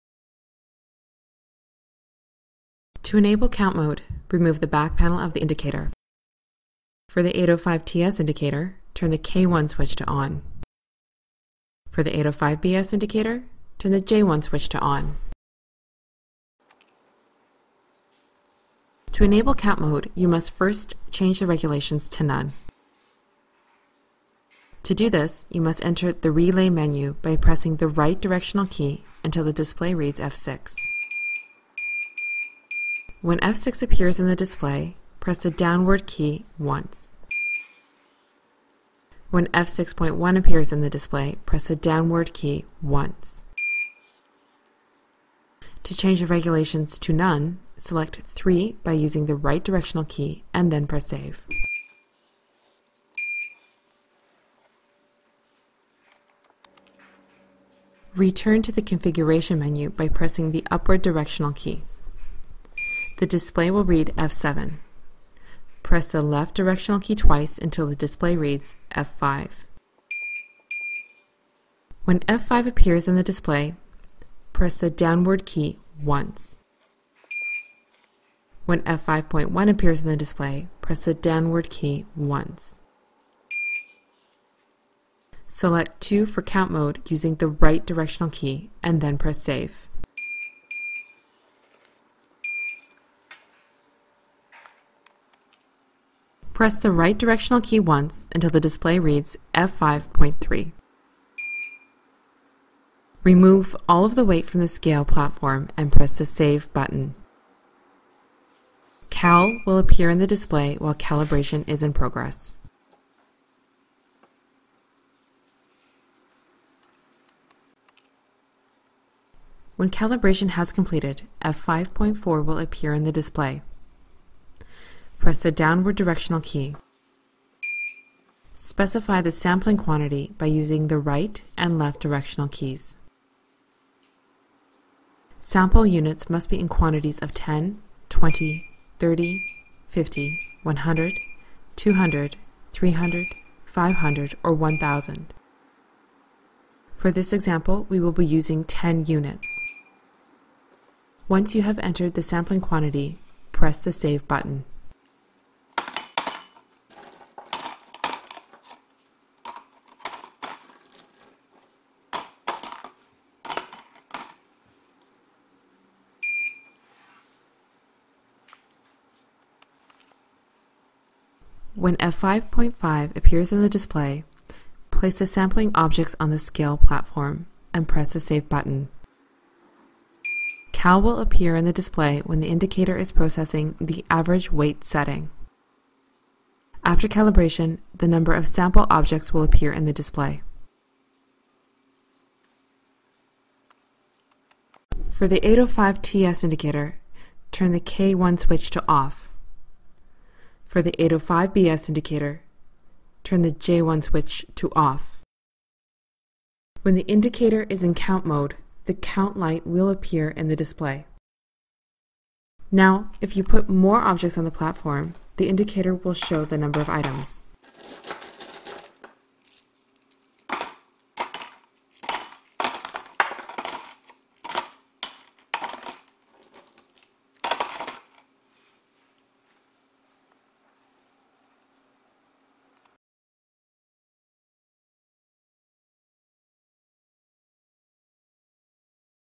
805TS and 805BS Series Digital Weight Indicators Count Tutorial
This video tutorial is applicable to all versions of 805TS and 805BS series indicators ( 805BS-B-16, 805BS-B-H, 805BS-B-17, 805BS-TN-B, 805TS-B or 805TS-B-16 and 805TS-B-17 ). Learn how to use the 'Count' function on the Anyload 805TS and 805BS Digital Weight Indicators.